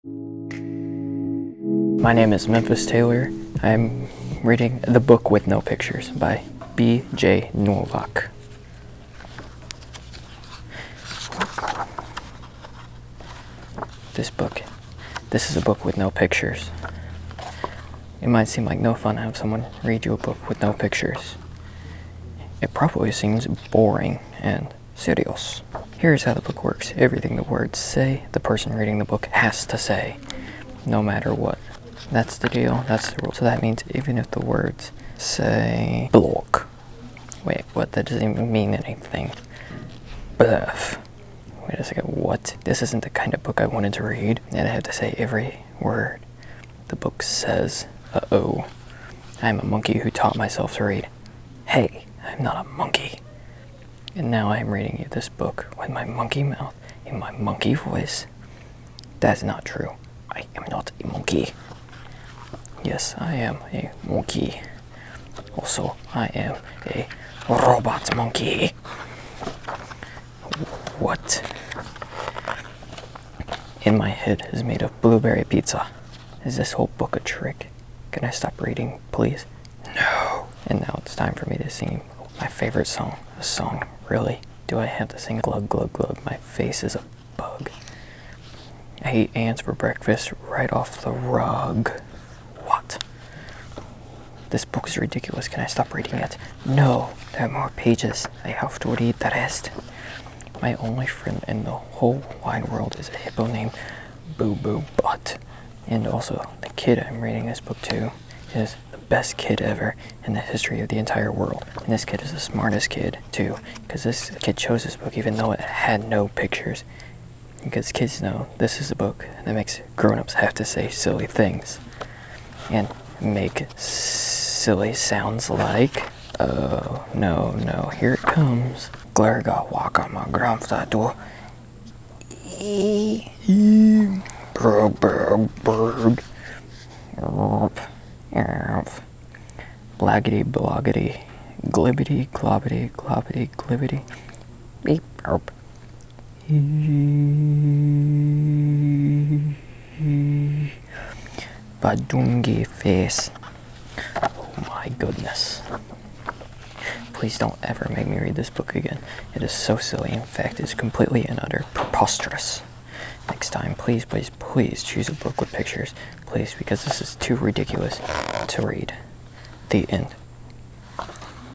Storytime with the Terriers- The Book with No Pictures by BJ Novak
Storytime with the Terriers- The Book with No Pictures  by BJ Novak. Read by Memphis Taylor.